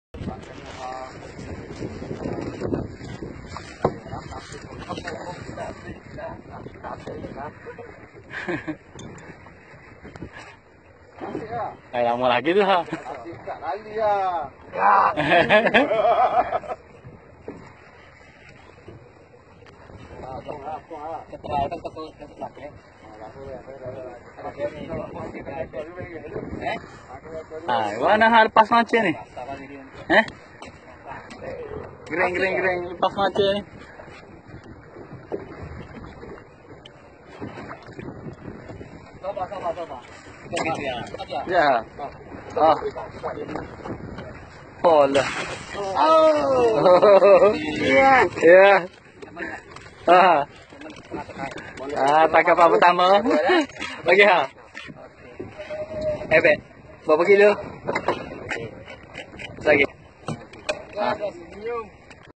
FISHING grup landak 2018 awsome big diamond trevaly fish 🐟🐟🐟
GRUP FISHING LANDAK 2018 IN ACTION
courtesy of Malaysian anglers fishing grup videos
by Mr imran taiko
using metal jig jigging deep sea fishing got big diamond trevaly fish 🐟🐟🐟🚢 on BOAT